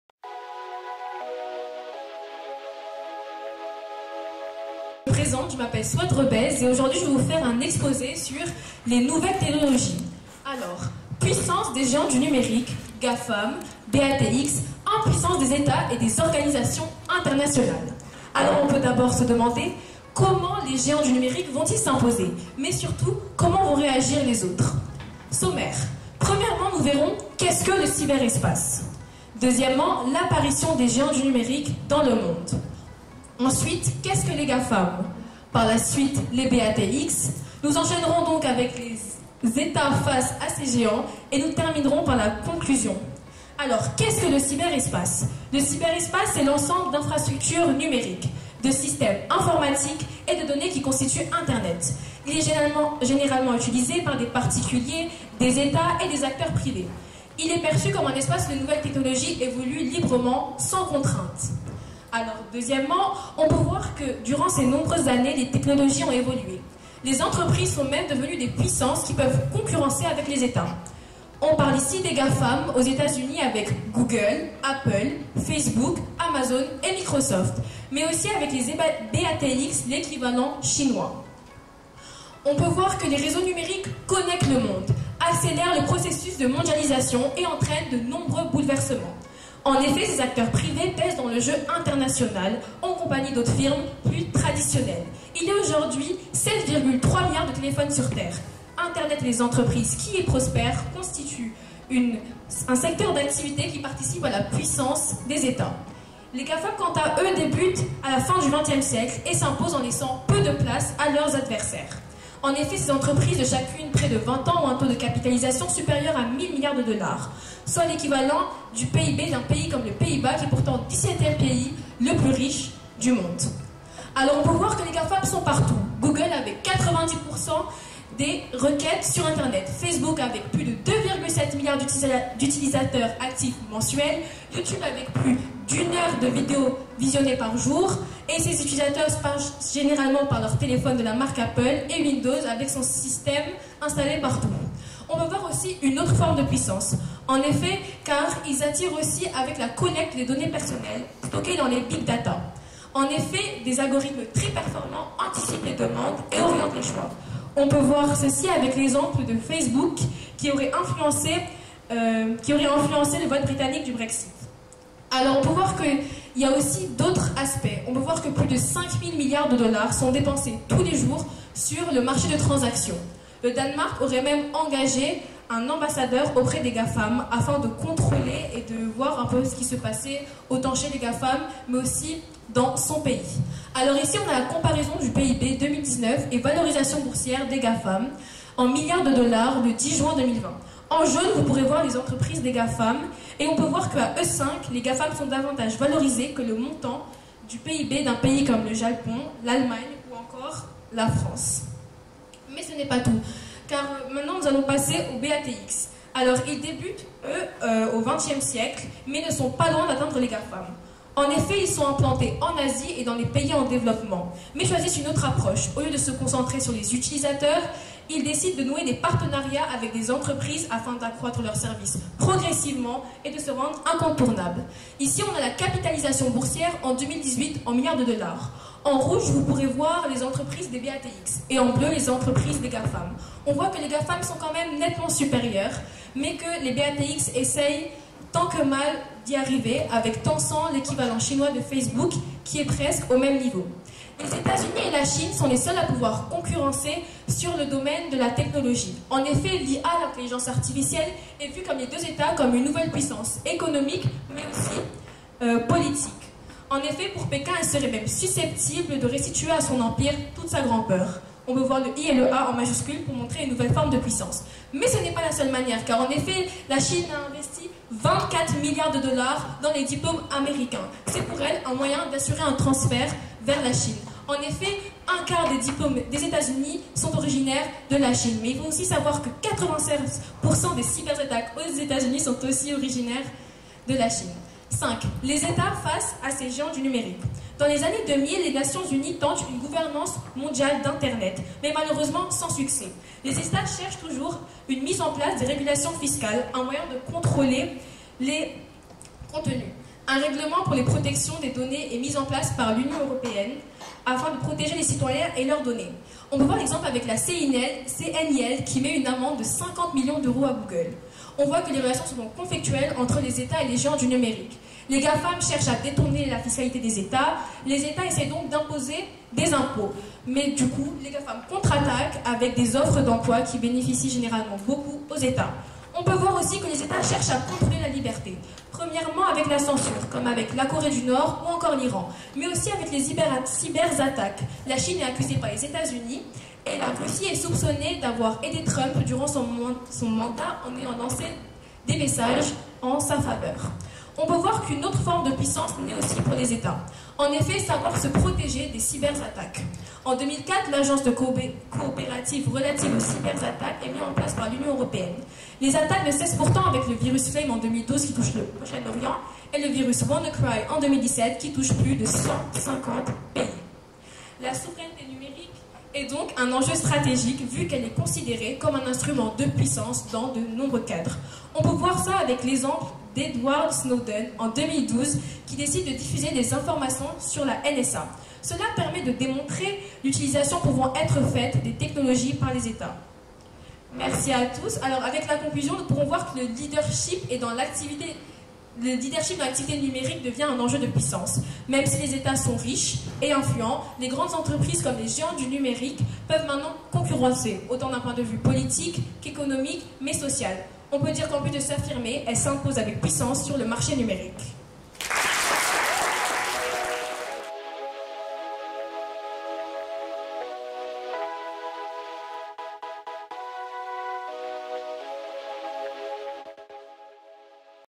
Pecha Kucha - Souad REBEIZ
Le "Pecha Kucha" - "bavardage" en Japonais - est une forme de présentation dynamique qui permet de présenter efficacement un sujet, une idée, un projet dans un laps de temps très limité et clairement défini : 6min 40s avec 20 diapositives de 20 secondes chacune. Tout un art qui permet de travailler aussi bien le fond que la forme en vue du grand oral de terminale !

Avec le groupe de 1ères spécialité HGGSP (Histoire, Géographie, Géopolitique et Sciences politiques), Mme Levet a organisé le "Pecha Kucha" durant les séances de classe. A l'heure de la finale, 3 candidates restent en lice.

Retrouvez  l'exposé de Souad REBEIZ sur "Les nouvelles technologies : puissance des géants du numérique (GAFAM, BATX, ...), impuissance des Etats et des organisations internationales ?".